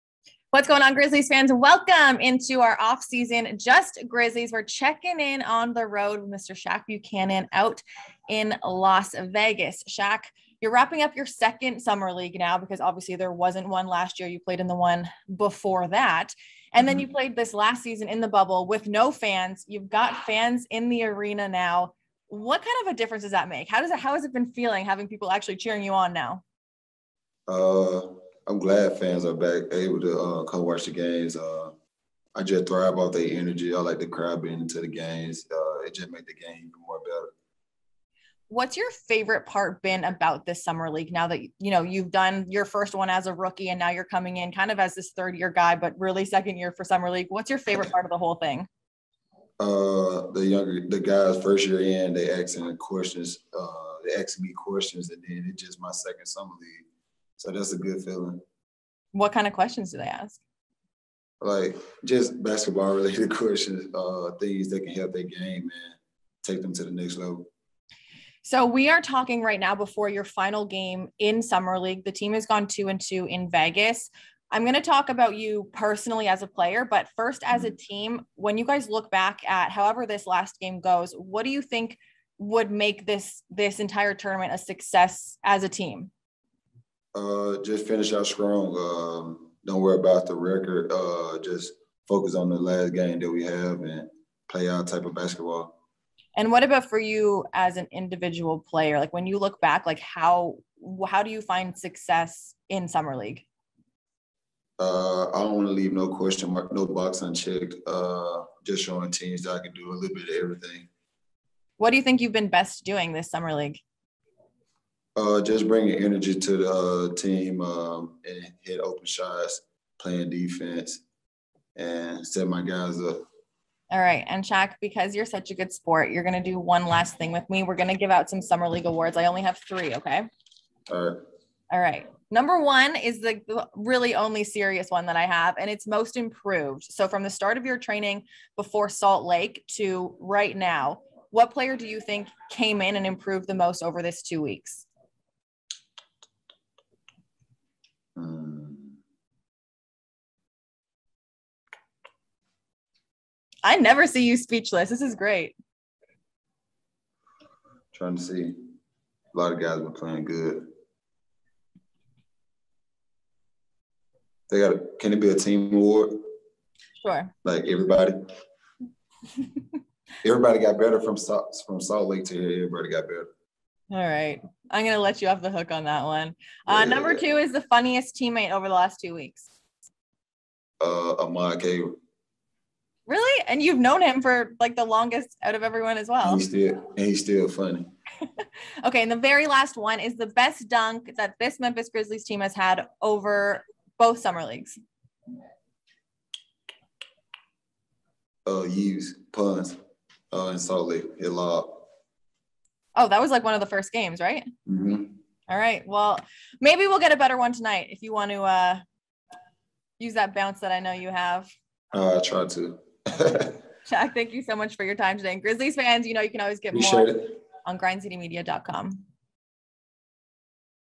Shaq Buchanan Talks NBA Summer League Experience! | JUST GRIZZLIES Podcast
Grind City Media's Kelcey Wright Johnson caught up with Shaq Buchanan ahead of tonight's NBA Summer League finale and talked about playing in front of fans, his favorite part of Summer League, views on success, and finally handing out team awards.

Listen to more behind-the-scenes chats with Grizzlies' players every other Thursday in the full-length audio version of JUST GRIZZLIES found on all your favorite streaming platforms